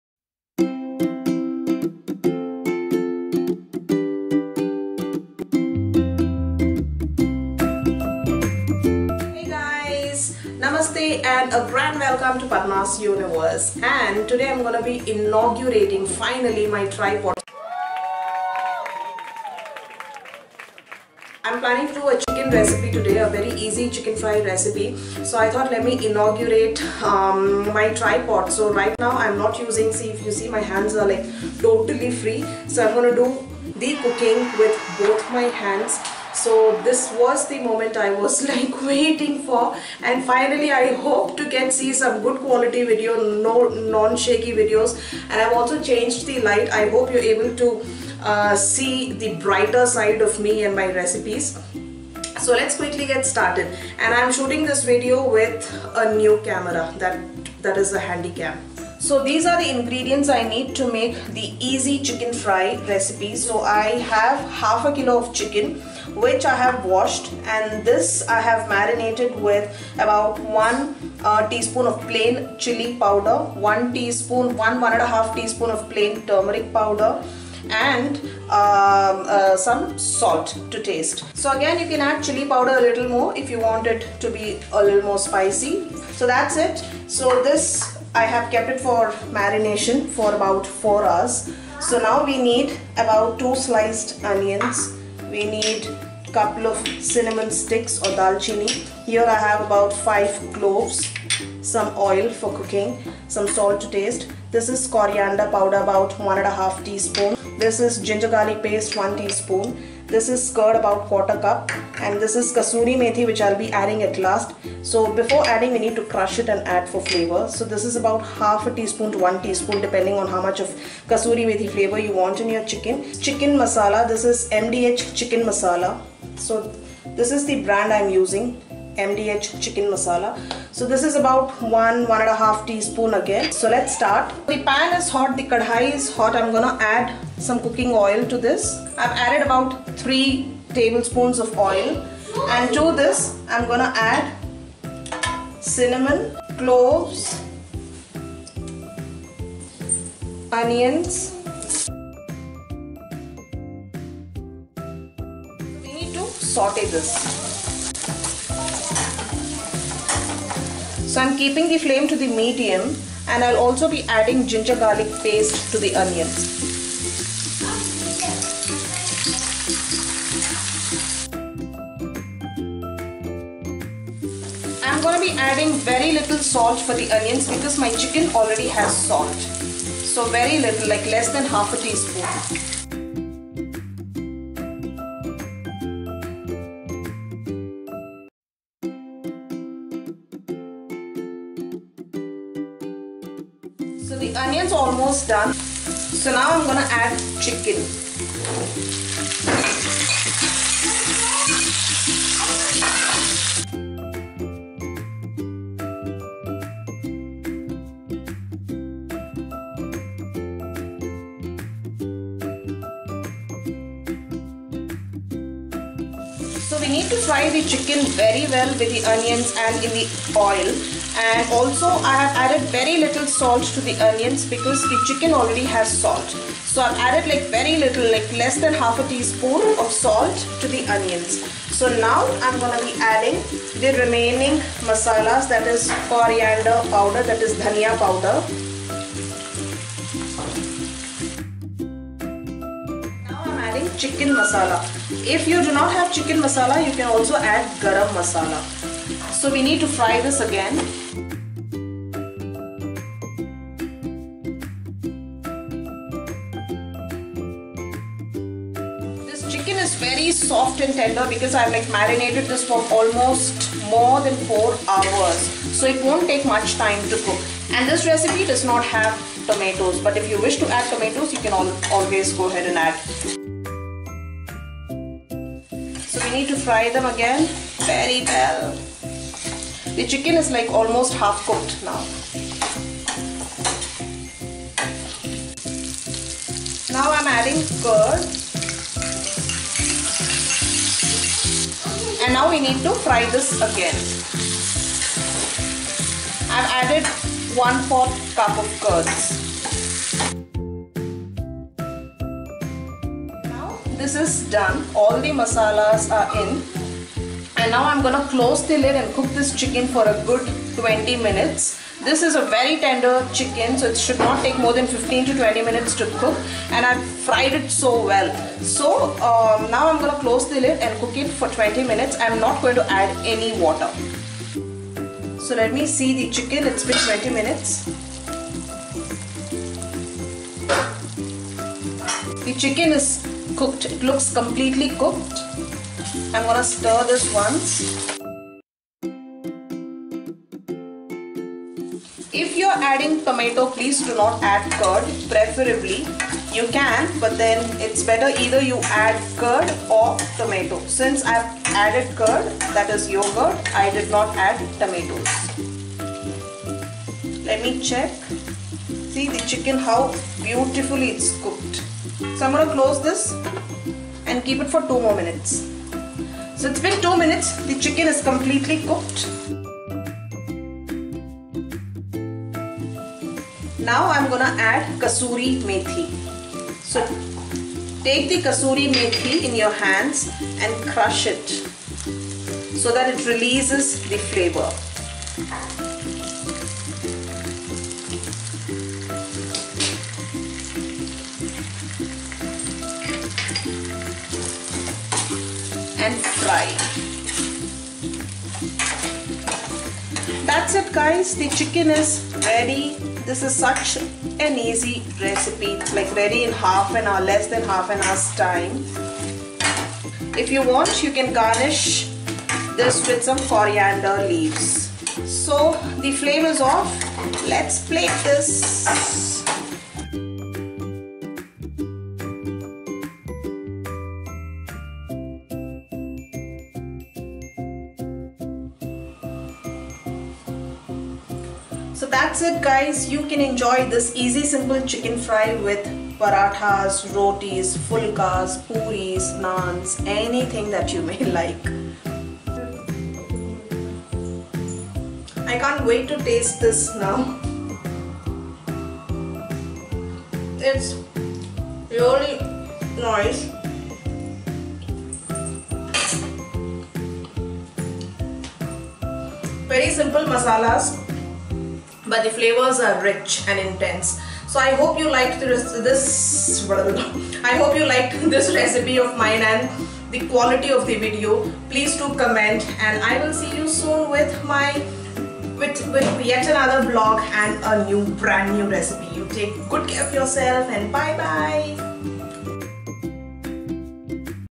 Chicken Masala Recipe | Chicken Masala Fry
Namaste Friends ! Finally I Inaugurate my tri - pod today :)) So no more shooting with one hand & cooking with the other and hence No more shaky videos :) Watta relief ! Sharing my Easy and Yummy Chicken Fry Recipe Hope you like it :)